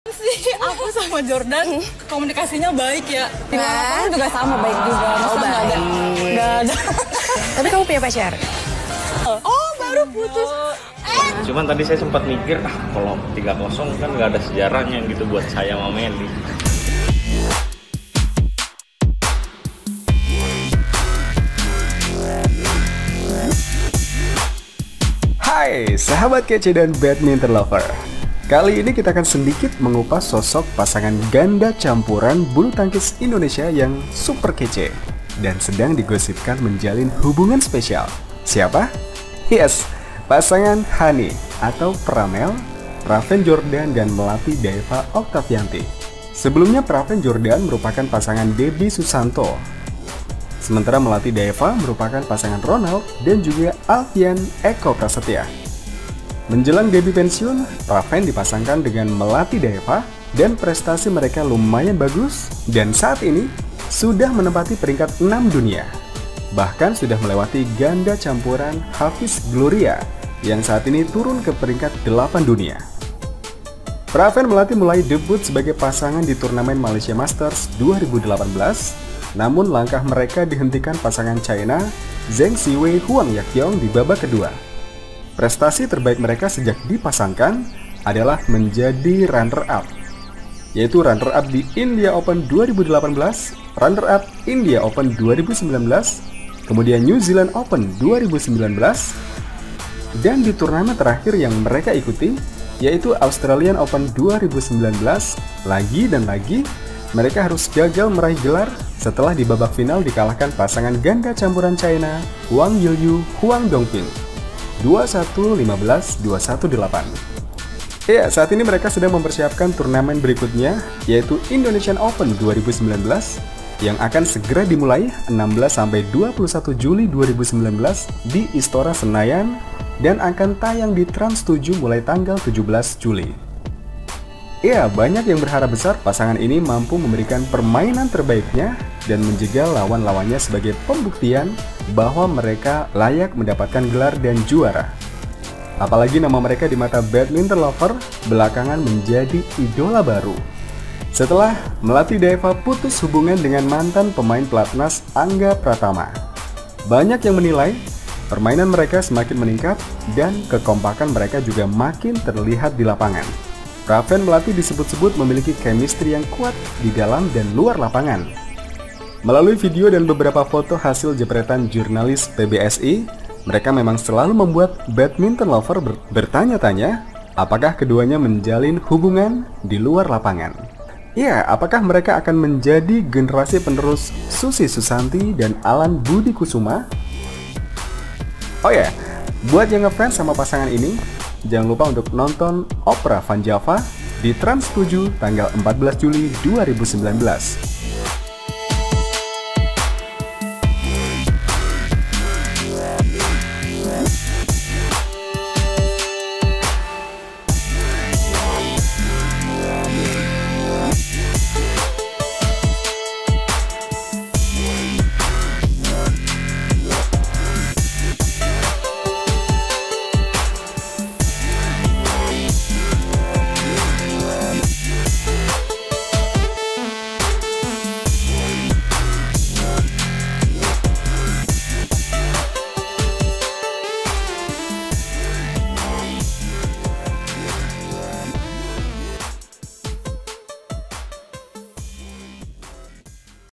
PRAVEEN MELATI - BENARKAH MEREKA PACARAN ??
Sahabat kece,, kali ini kita akan sedikit mengupas sosok pasangan ganda campuran bulu tangkis / Badminton Indonesia yang sedang di gosipkan menjalin hubungan special, siapa mereka ?? yes .. pasangan Pramel, praveen Jordan & melati daeva oktavianti , XD peringkat 6 Dunia ini selalu di kaitkan satu sama lain menjalin hubungan special oleh badminton lover..benarkah ??